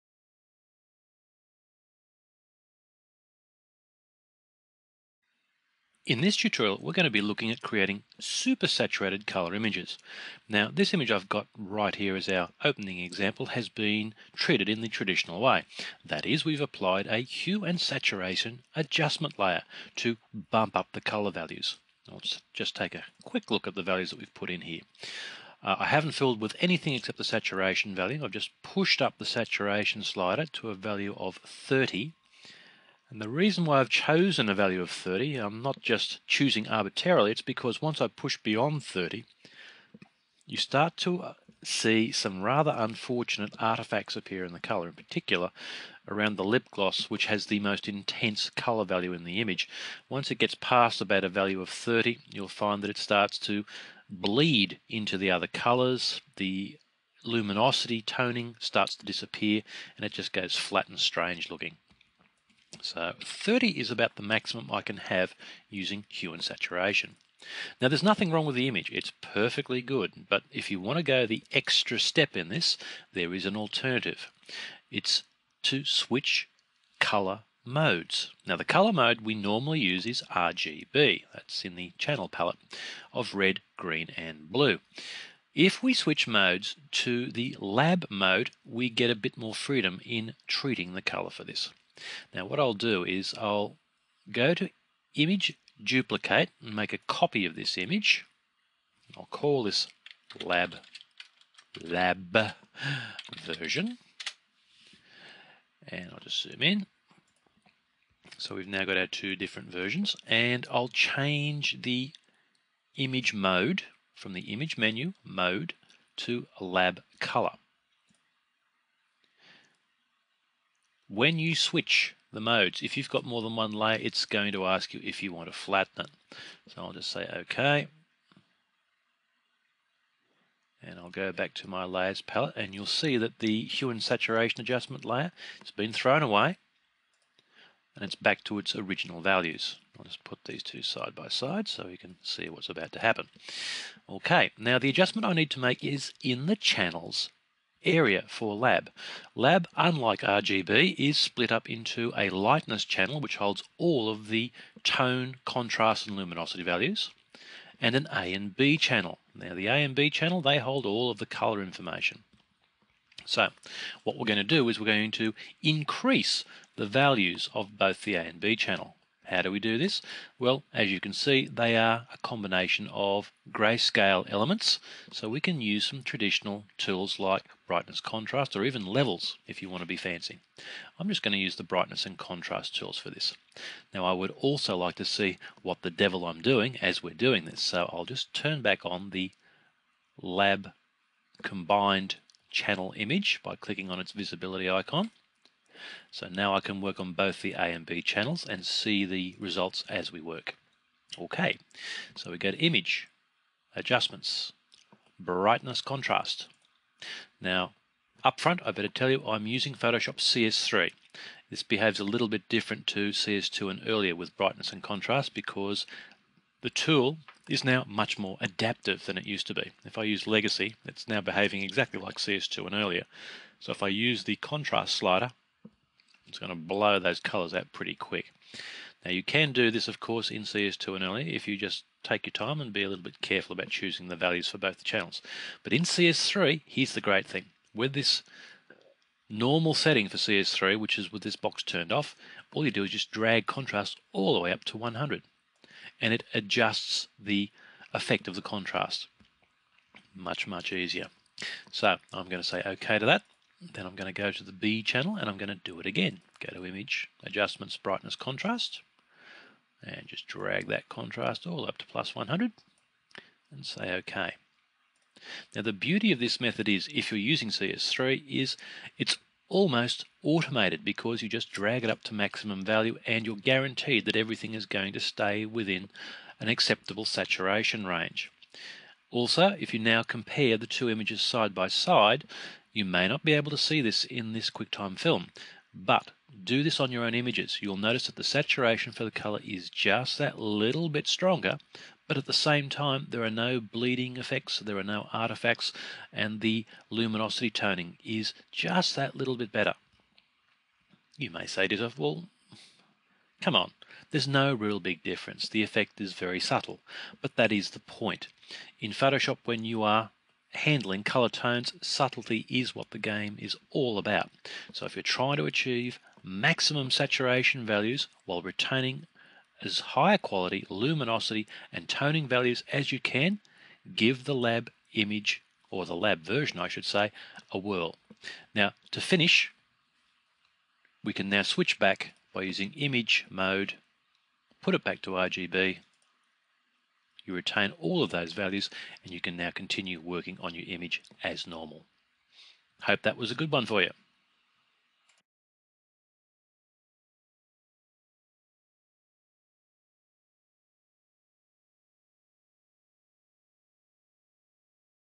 Lab color for super saturation: Photoshop retouching:
Get some really intense super rich color by giving Lab Color mode a spin! Easy and healthy too.